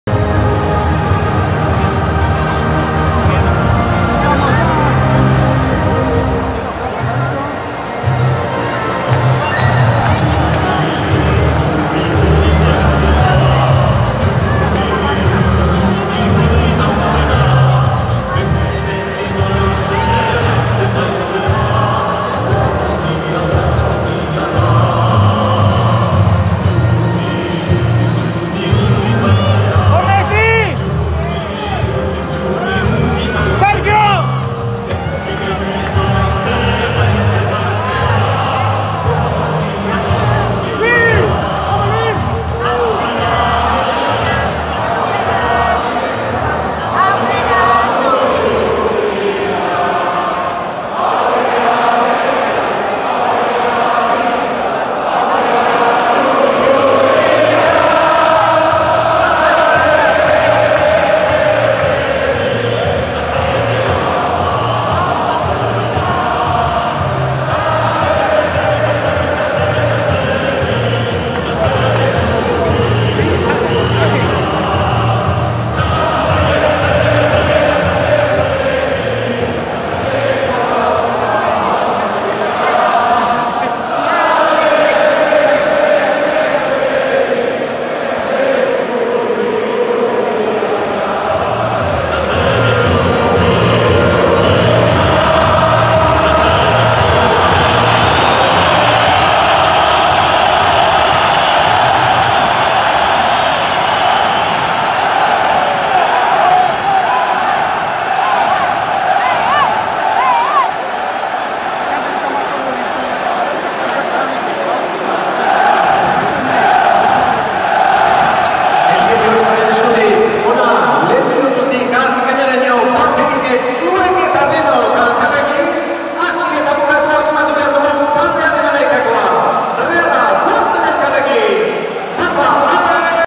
REAL SOCIEDAD v BARCELONA PRE MATCH
Pre Match Atmosphere of Real Sociedad v Barcelona in December 2019.